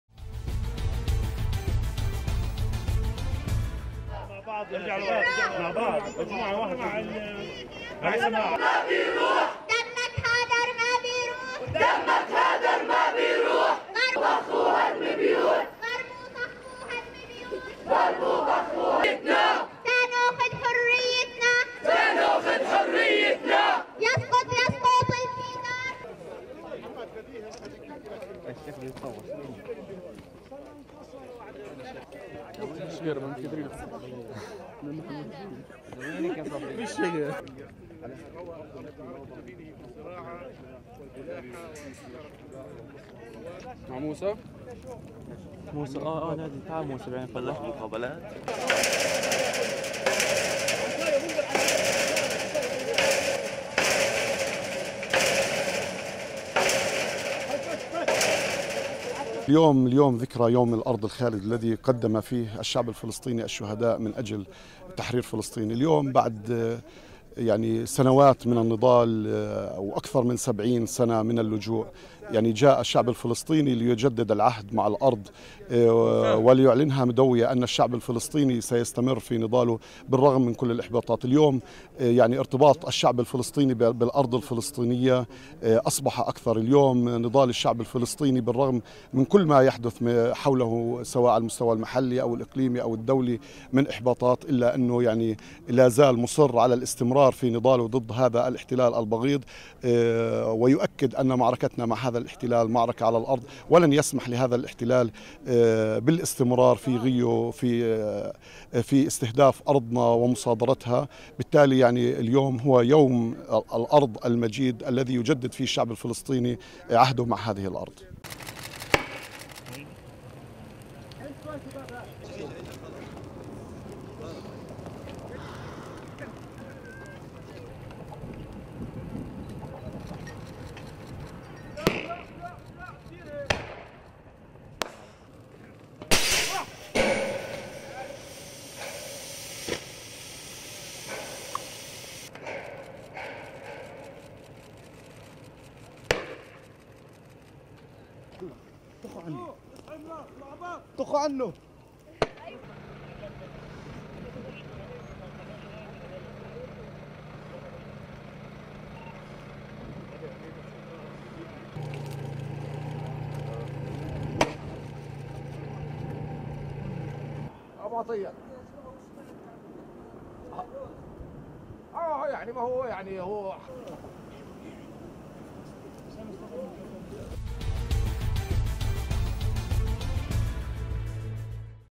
مصر العربية | الجيش الإسرائيلي يفرق مسيرة فلسطينية في ذكرى يوم "الأرض"
فرق الجيش الإسرائيلي، اليوم الخميس، مسيرة نظمها فلسطينيون، في مدينة بلدة بيت جالا جنوبي الضفة الغربية المحتلة، في الذكرى الـ"41" ليوم "الأرض".